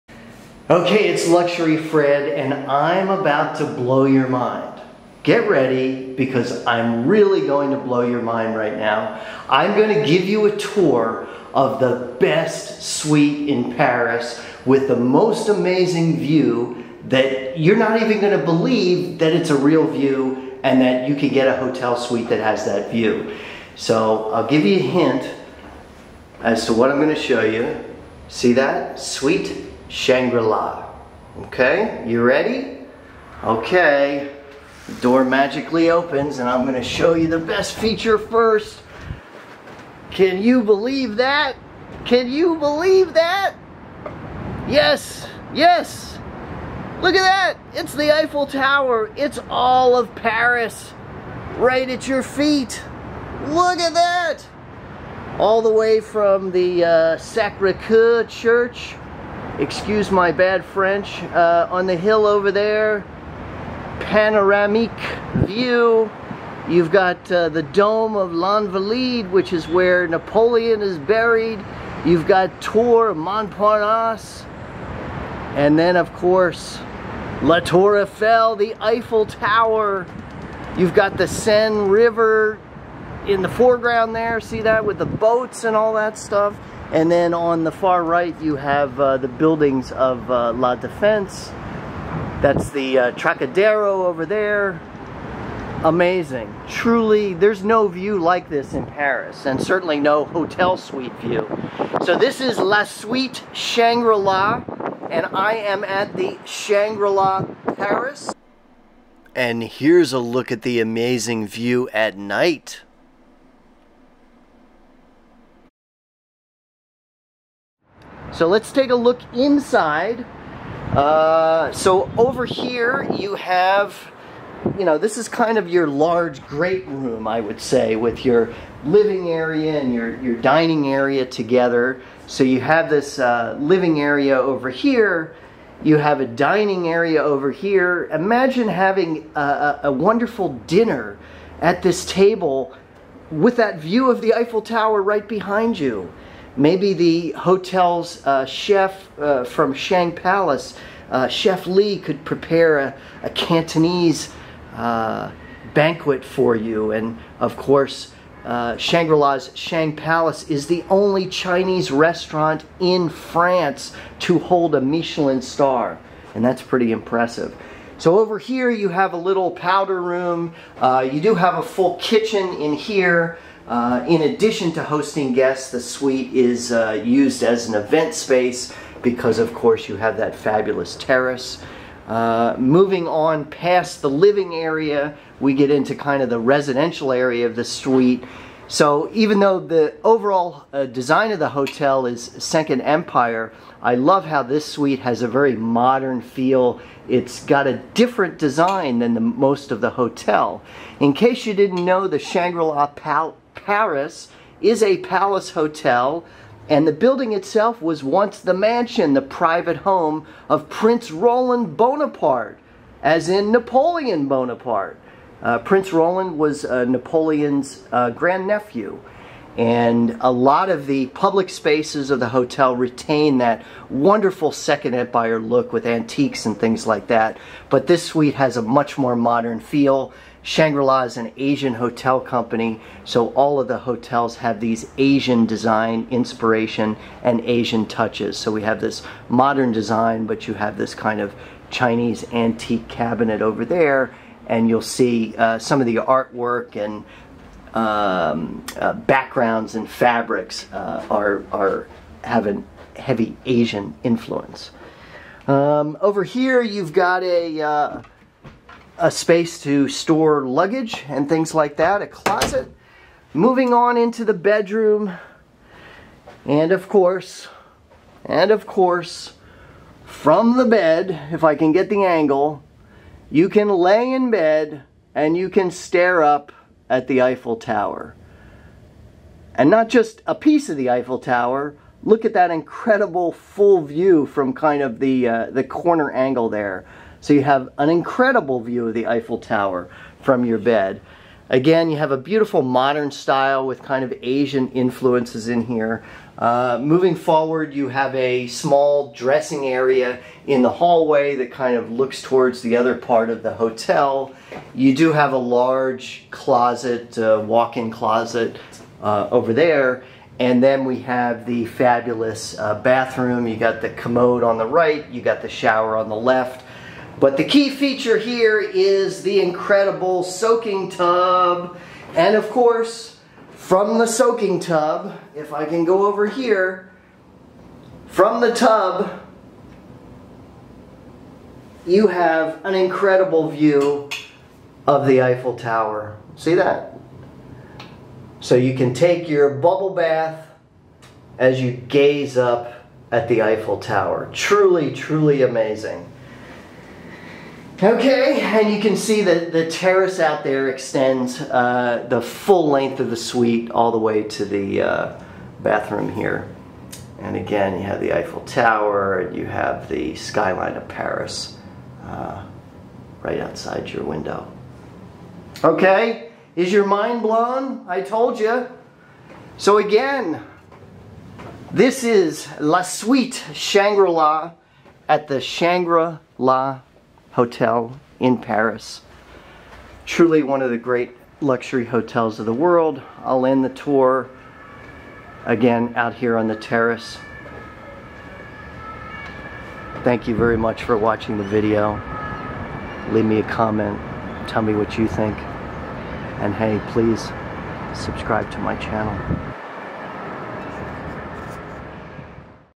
TOUR THE BEST HOTEL SUITE IN PARIS - LA SUITE SHANGRI-LA at SHANGRI-LA PARIS
Are you ready to be amazed? Here is a full tour of the incredible Shangri-La Suite at the Shangri-La Paris. Check out the jaw-dropping views of the Eiffel Tower from the huge terrace (or from the bed or from the bathtub!!). The Shangri-La Suite has 120 sqm / 1,290 square feet of transcendent luxury with an enormous private, furnished 100 sqm / 1,076 square foot terrace offering a bird’s-eye view of Paris’ most iconic sights.

Perched on the top floor of the luxury Paris hotel, the showstopping La Suite Shangri-La is a modern apartment in the sky boasting unobstructed views of the iconic Eiffel Tower. Carefully balancing classic French Directoire and Empire styles with modern Art Deco details, this one-bedroom suite features a walk-in dressing room, separate living room and dining area, functional kitchenette, guest bathroom and the pièce de résistance – a grand terrace offering sweeping views of the City of Lights. You get truly unparalleled panoramic views from the Eiffel Tower to Le Grand Palais and Sacré-Coeur.Custom furnishings including a hand-painted chinoiserie cabinet, Chinese antiques, pearlescent Lelièvre wall panels and Manuel Canovas drapes. It has a residential feel and feels more like staying at a friend's luxurious Paris apartment than staying in a hotel room.

La Suite Shangri-La has a beautiful modern style with the Asian accents typical to Shangri-La hotels. It's a nice contrast with the style of the rest of the hotel, the public spaces and guest rooms, which have a classic, Second Empire style due to the hotel's history.

Built in the late 19th century, the Shangri-La Paris building formerly was the private mansion of Prince Roland Bonaparte, grand-nephew of Napoleon. The public spaces of the Paris palace hotel retain many historical details and architectural elements. Check out my video tour of the Shangri-La Paris lobby to see these elements and features.